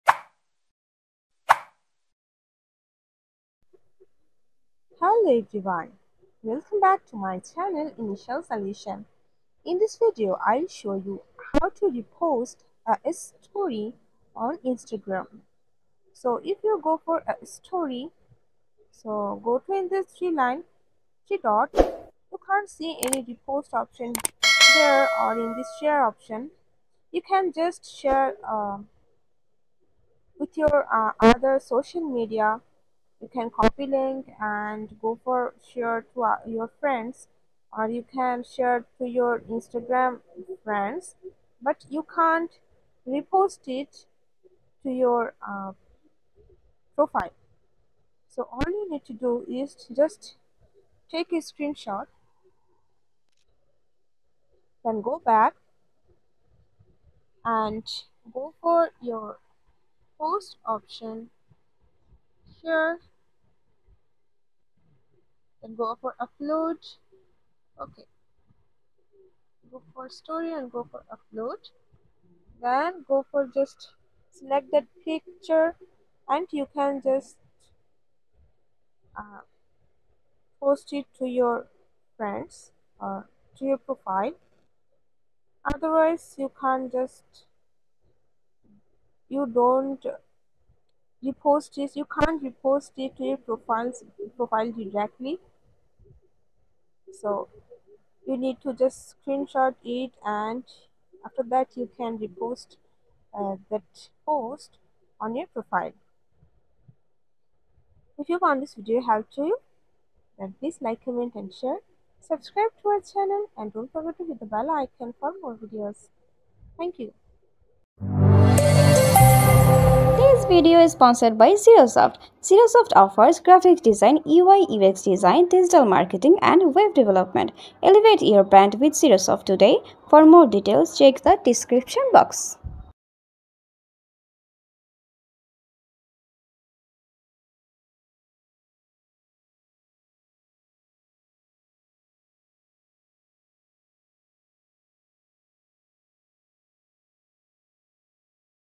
How to Repost a Story on Instagram 2024 | Share Your Favorites with Ease! 📸🔄
🌐💡 Welcome to Initial Solution, your go-to destination for all things social media savvy! In today's video, we're diving into the world of Instagram and showing you How to Repost a Story on Instagram. Whether it's a friend's hilarious moment or a noteworthy post, we've got you covered. 📸🔄

📌 Here's what we'll cover in this step-by-step guide:
Step 1: Why Reposting Stories Matters
Step 2: Launching the Instagram App
Step 3: Navigating to the Story You Want to Repost
Step 4: The Reposting Process
Step 5: Customizing Your Repost

📖 Chapters of "How to Repost a Story on Instagram":
0:00 Introduction
0:12 How to Repost a Story on Instagram
1:08 go first story and go for upload
1:35 screenshot and repost
1:46 Conclusion
2:01 Sponsored Messages

🌟 Ready to become an Instagram story reposting pro? Let's get reposting and sharing those favorite moments effortlessly! 📸🔄